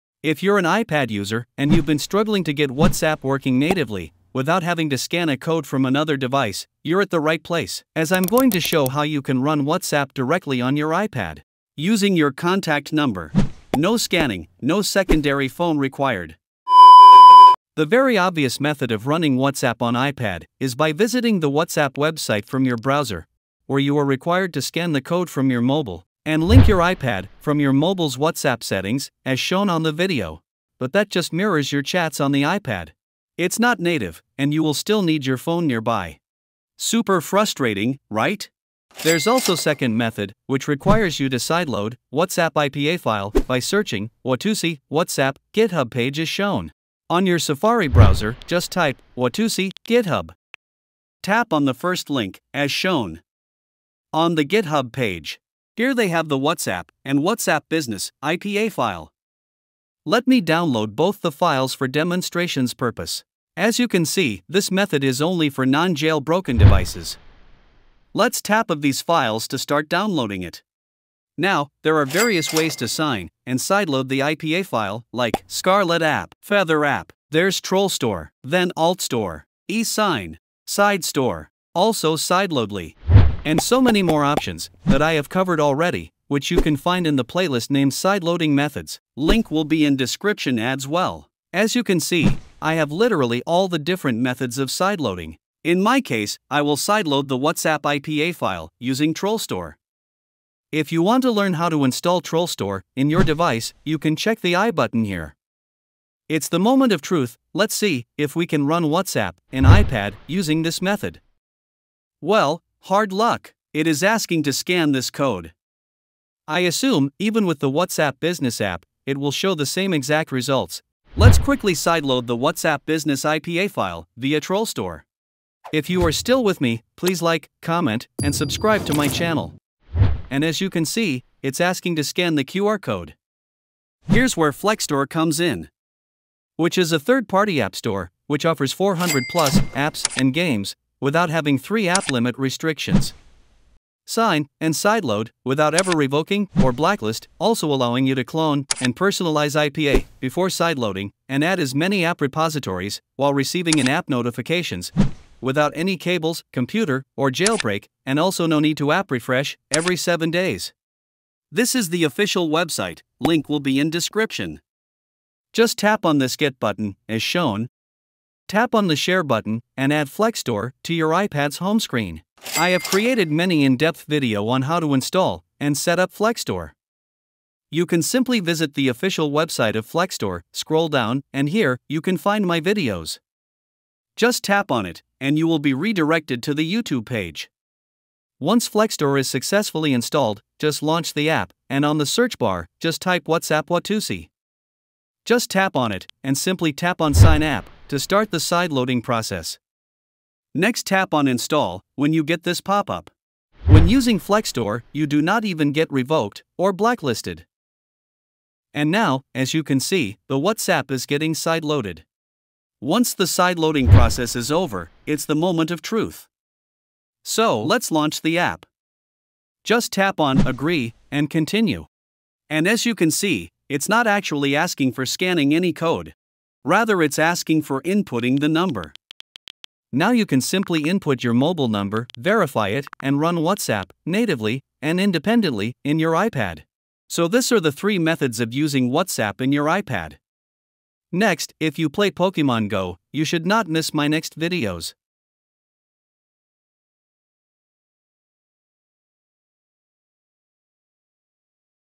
WhatsApp for iPad: No Phone, No Scan Required (Run Natively)
Run WhatsApp on iPad natively in 2024! No phone, no scan! Learn how to install WhatsApp on iPad with your number.

Looking to run WhatsApp natively on your iPad in 2024? Say goodbye to QR codes and links! Learn how to install WhatsApp on iPad Pro or any iPad without needing your phone. We’ll walk you through using WhatsApp with your number. Download WhatsApp for iPad today and finally use it seamlessly. Watch now and start using WhatsApp on your iPad natively like never before!

 💬 Lets Join Over My Social Handles 💬